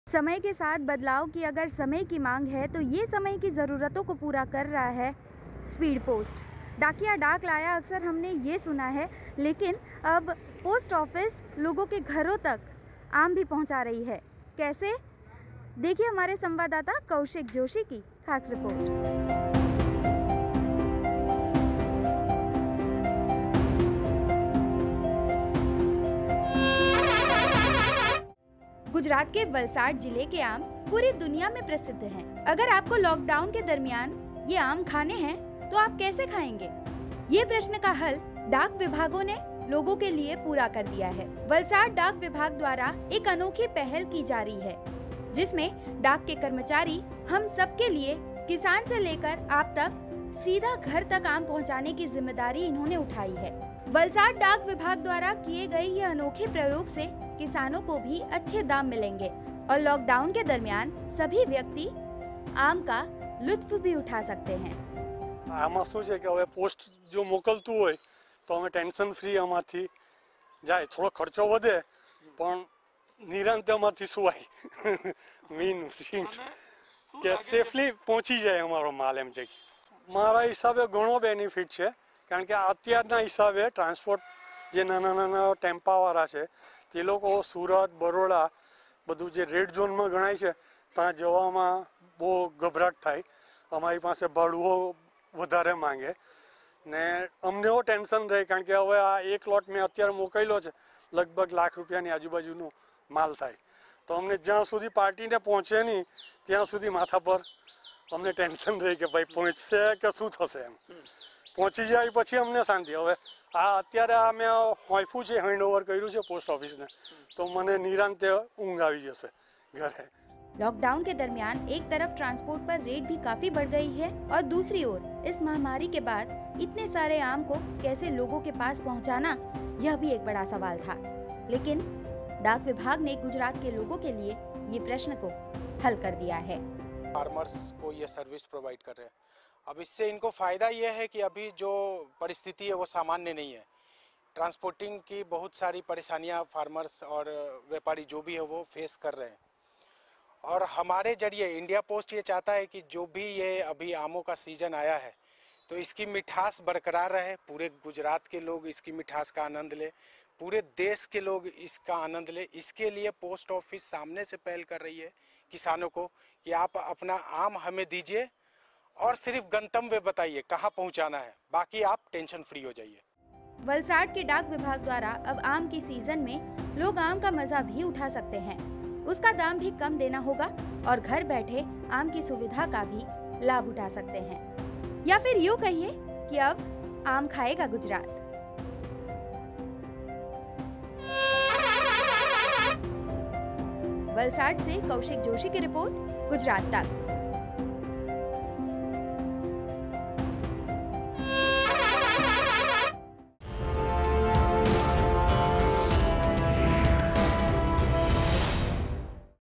Lockdown 4.O : Speed Post से Speed में पहुंचेंगे आम ! Gujarat tak
क्या आपको आम खाना है यह आम कैसे आएगा कौन लाएगा यह सवाल आपको शायद सता रहा होगा लेकिन यह आपकी खुशी अब डाक विभाग द्वारा आपको पूरी की जाएगी जी हां अब डाक विभाग किसान लेकर आपके घर तक डाक विभाग पहुंचाएंगे आम.